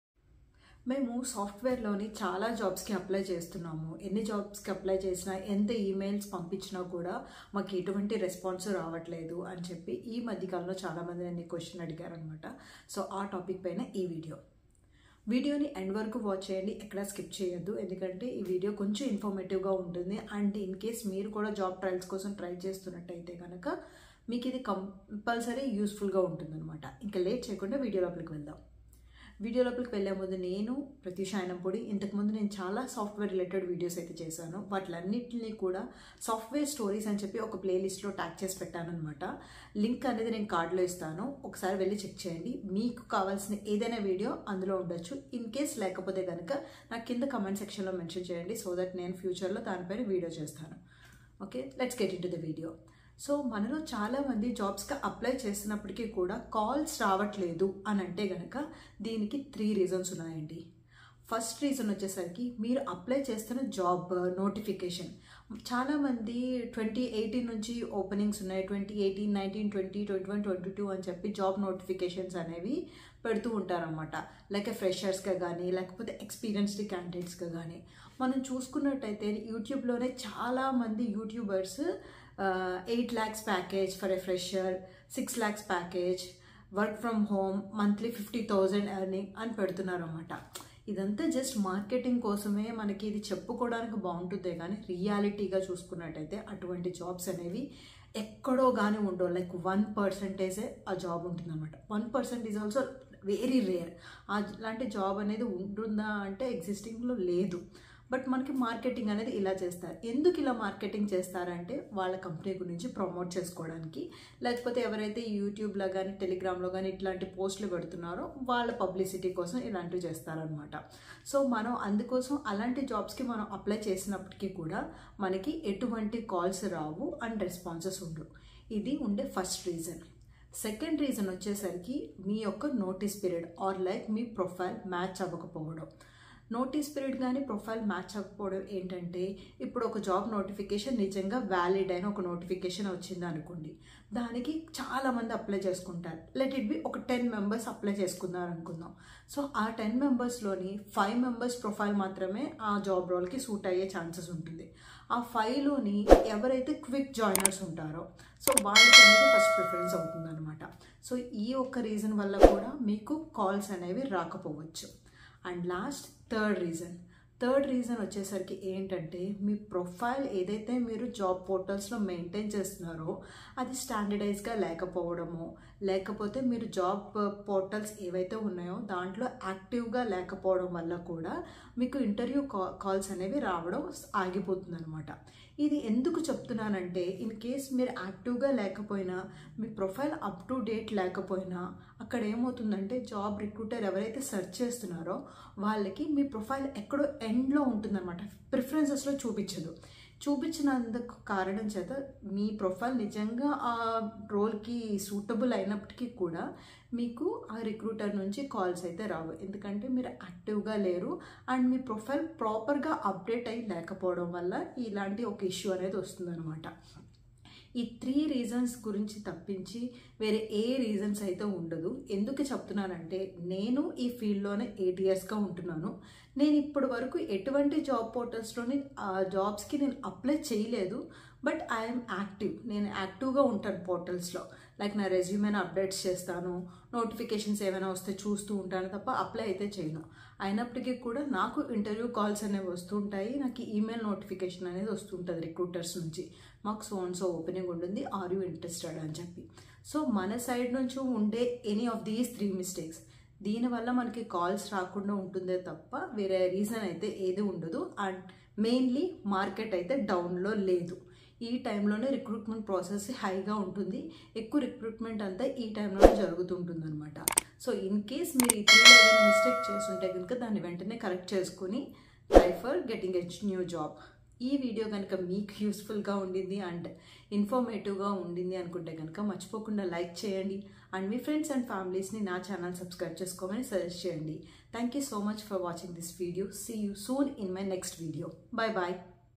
Apply for IT jobs but no interview calls (Telugu) | @VoiceofSoftware
everyone is complaining about that they are nit receiving the interview calls though they applied for many jobs, it doesn't mean that IT is down, we might did some mistake from our side. watch this video to clarify on the same.